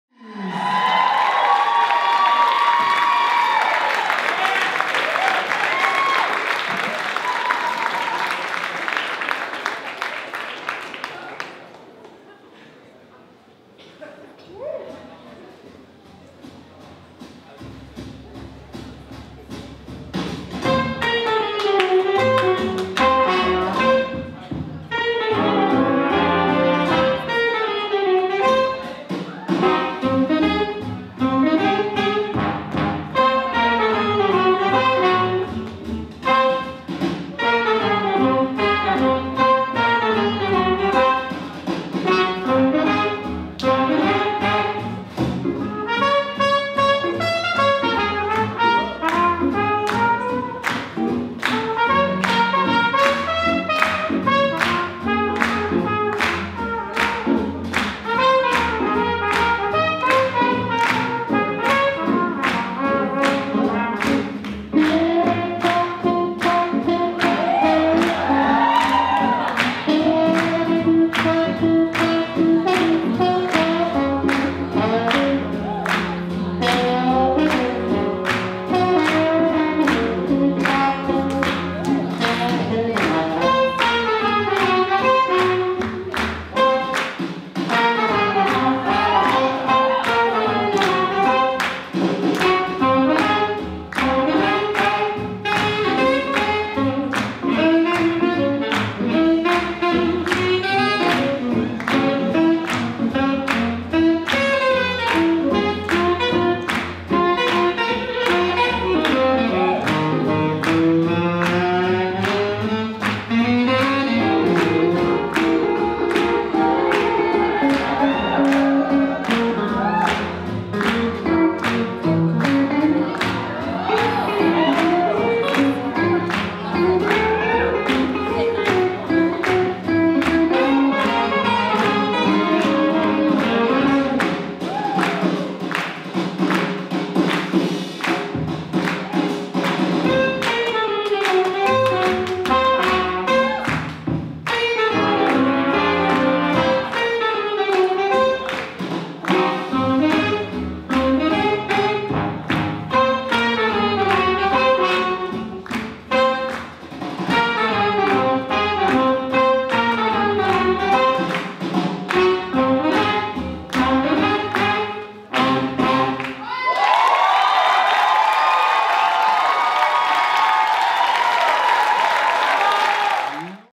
DTF 2017 - Laura Glaess @ Friday Night Show
Friday Night Show 2017
Performance solo / vernacular jazz
par Laura Glaess
Dig Tha' Feet - 6e édition - 27 au 29 janvier 2017
Théâtre Plaza - Festival de danse et de musique à Montréal (Canada)
Jive / Boogie Woogie / Lindy Hop